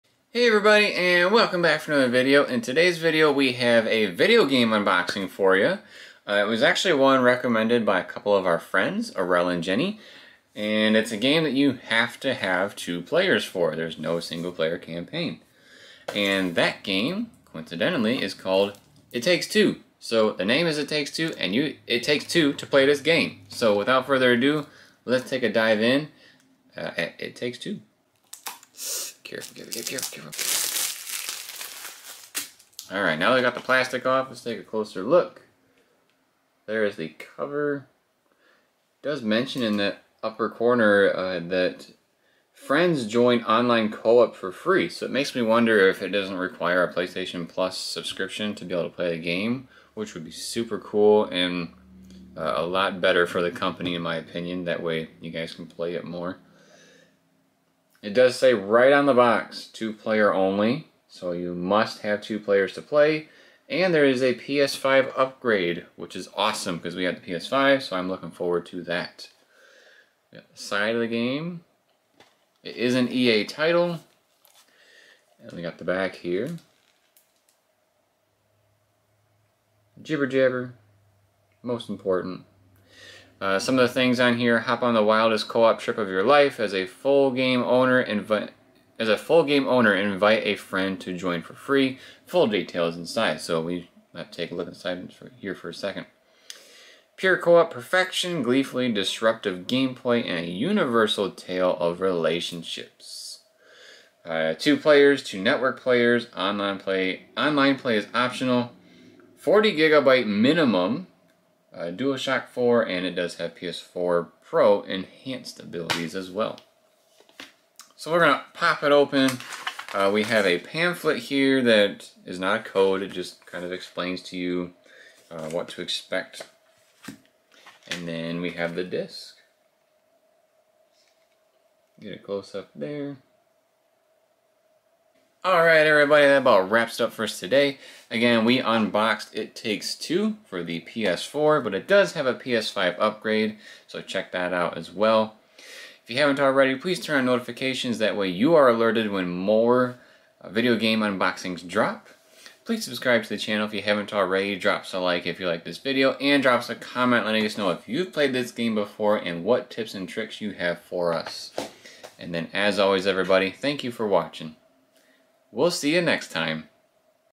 It Takes Two Unboxing!!!
In today's video, we unbox It Takes Two for the PS4!!!